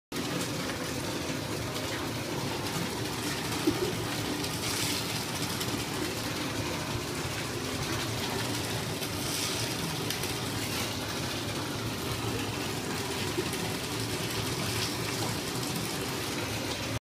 Guinea pigs take a bath
Two American Guinea pigs take a bath.